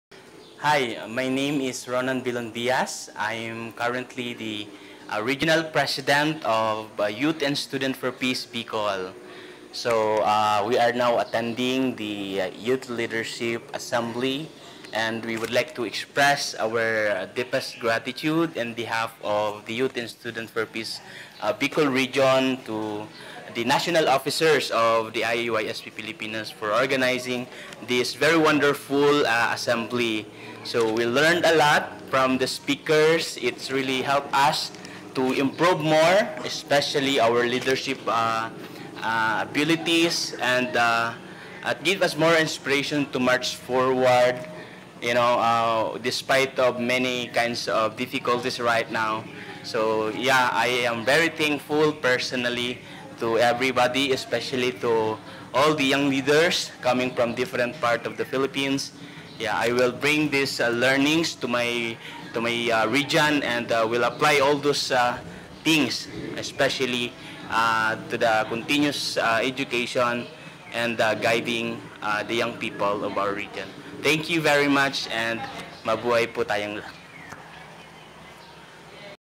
Testimonies | Youth Leaders Assembly 2022
𝗪𝗮𝘁𝗰𝗵 | Mr. Ronan Bilon Bias, President of the IAYSP Bicol Chapter, expressed his gratitude for organizing the IAYSP Youth Leaders Assembly, stating that they learned a lot and gained more knowledge in order to improve and develop their leadership skills.